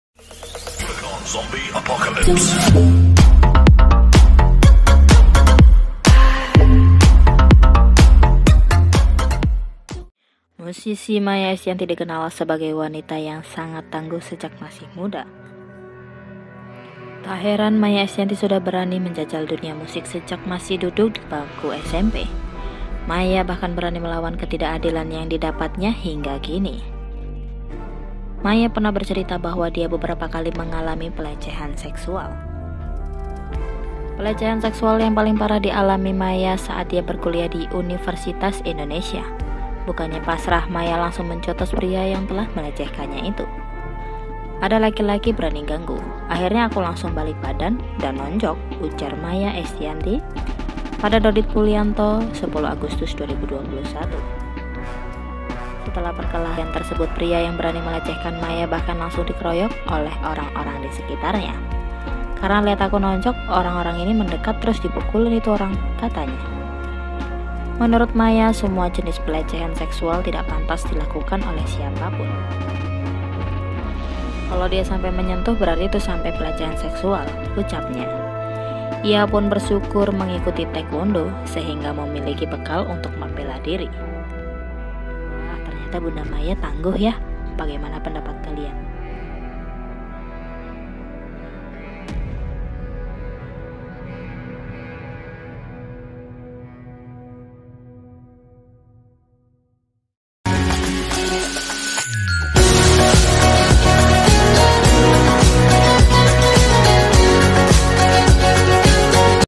Tangguh, Maia Estianty Pernah Tonjok Orang Yang Berani Mengganggunya
Musisi Maia Estianty dikenal sebagai wanita yang sangat tangguh sejak masih muda. Tak heran, Maia Estianty sudah berani menjajal dunia musik sejak masih duduk di bangku SMP.